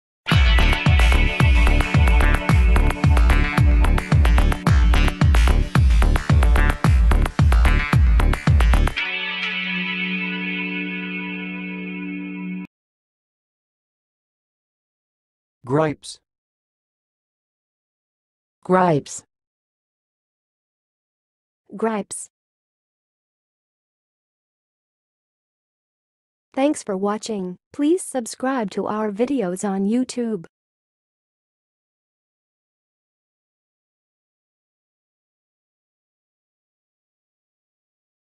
How to say "gripe's"! (High Quality Voices)
Watch in this video how to say and pronounce "gripe's"!